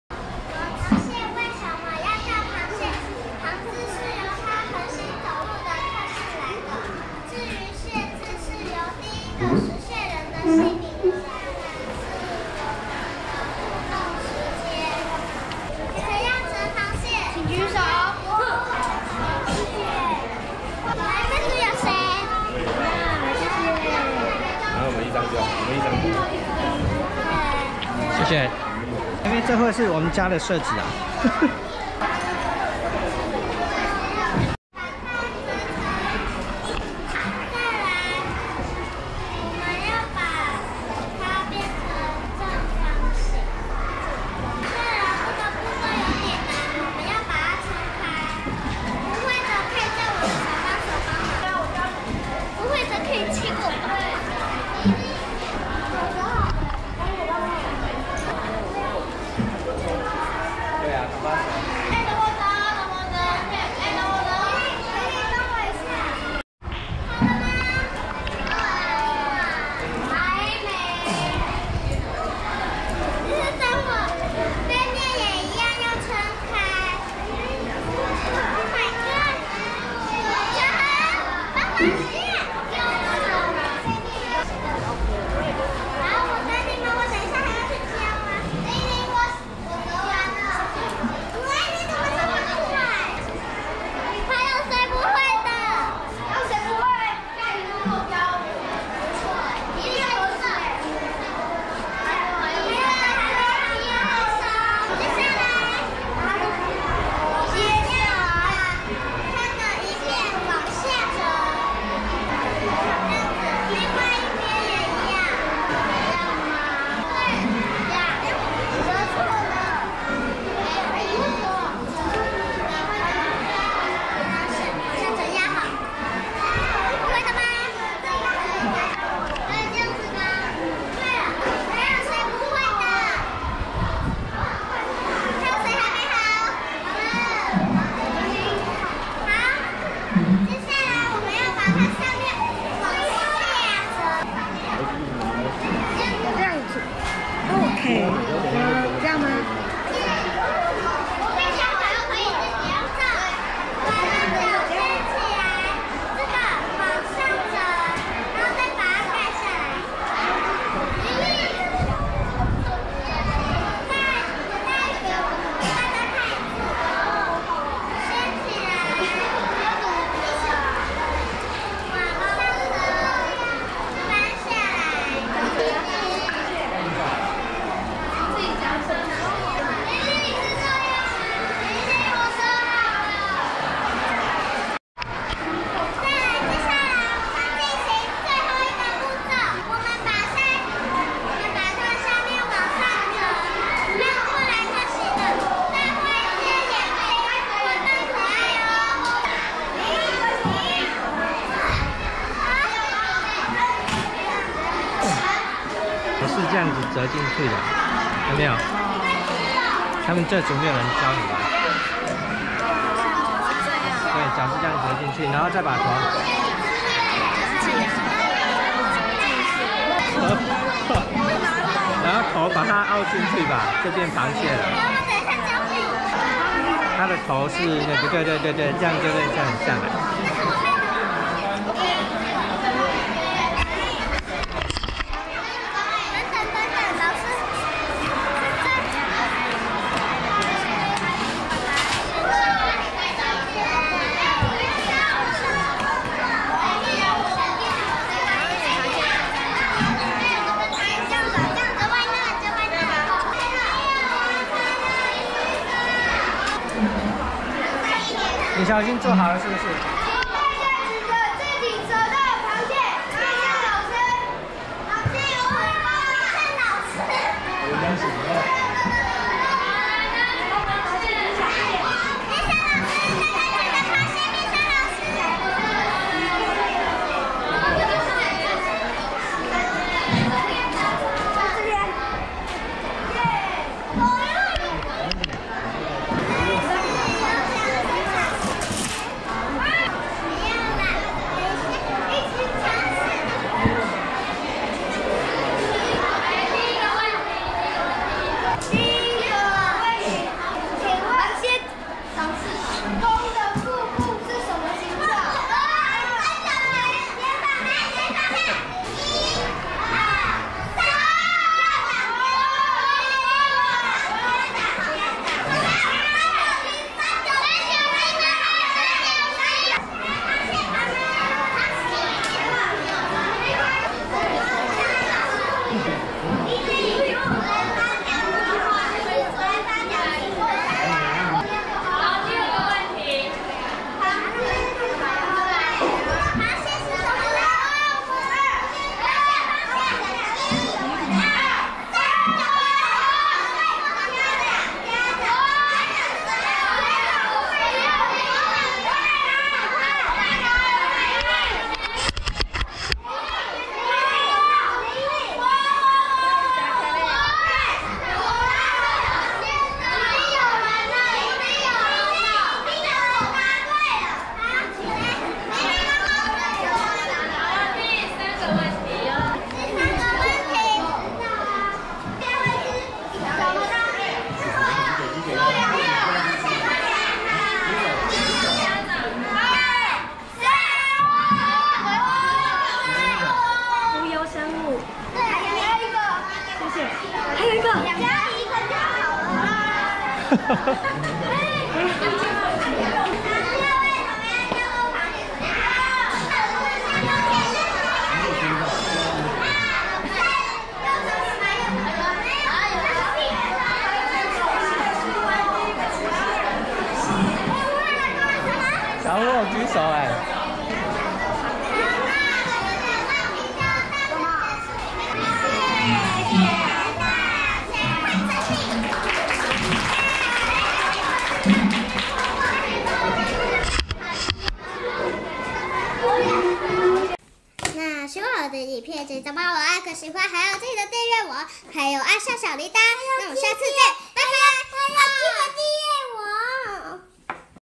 2019年5月25日_深美國小成果展205班_(六)螃蟹吐泡泡(下)小狐教大家摺螃蟹
成果展下半場換小狐教大家摺螃蟹.